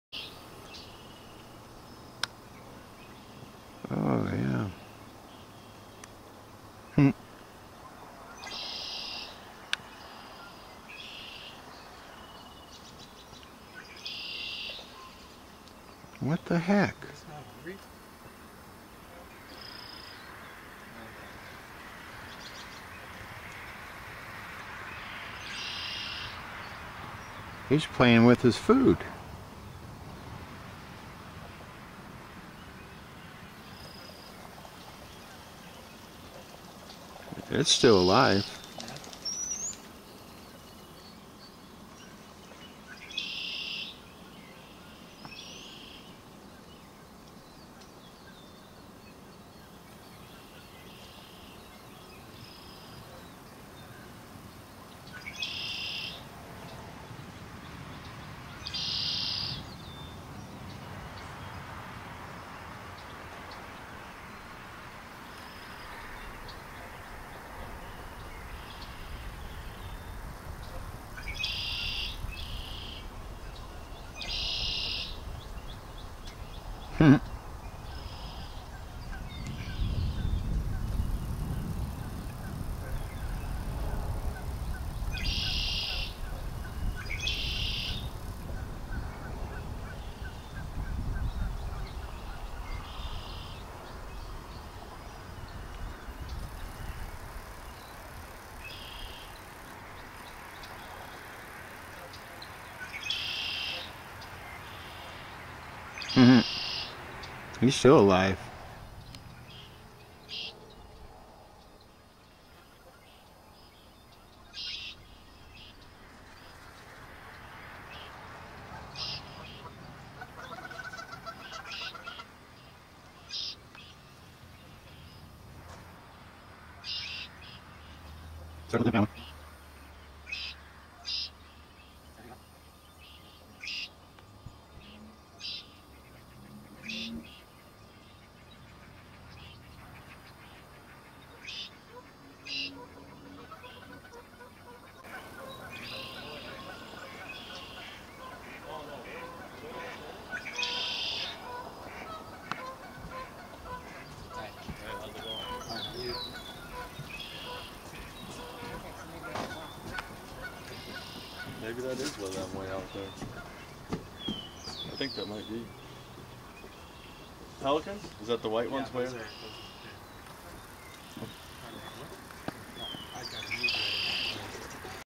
Nygren egret eating frog
I had to speed part of this up because it takes about 5 mins. for them to kill and eat a frog.